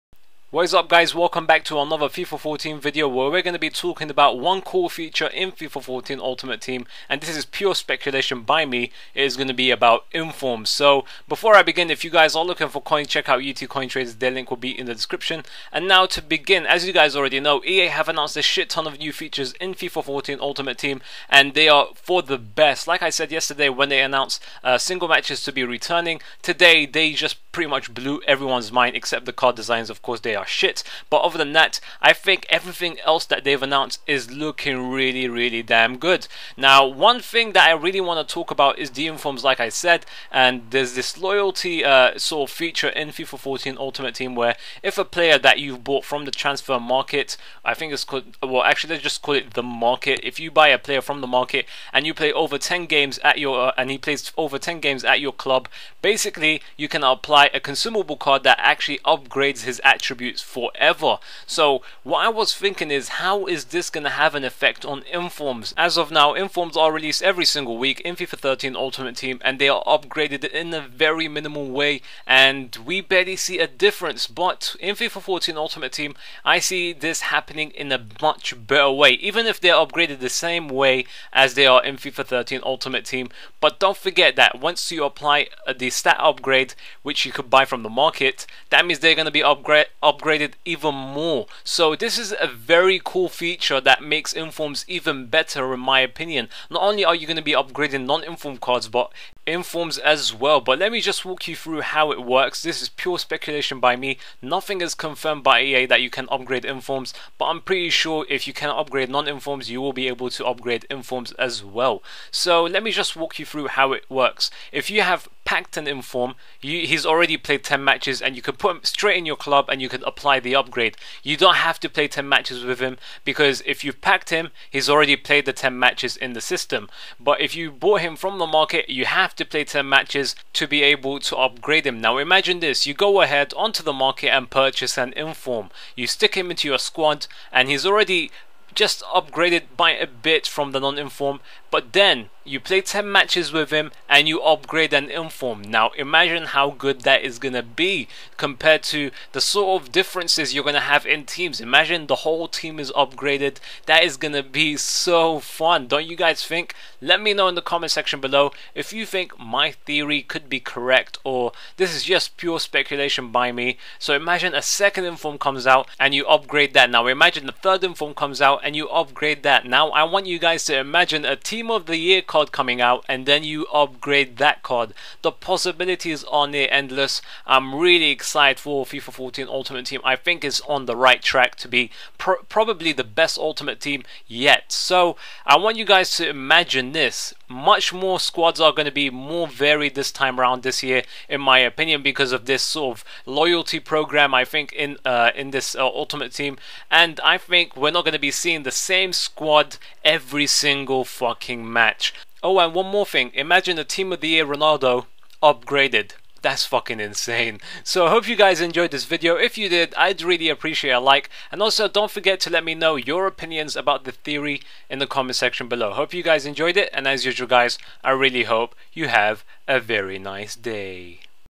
FIFA 13 UT FIFA 14 Ultimate Team Informs Theory
fifa 14 ultimate team news was released by EA but not match was talked about in regards to informs in fifa 14 ultimate team. So here is a theory about loyal informs in fifa 14 ultimate team that play over 10 matches at your club you will be able to upgrade them in my opinion or even fifa 14 toty cards. Let me know your opinion aobut this in the comment section.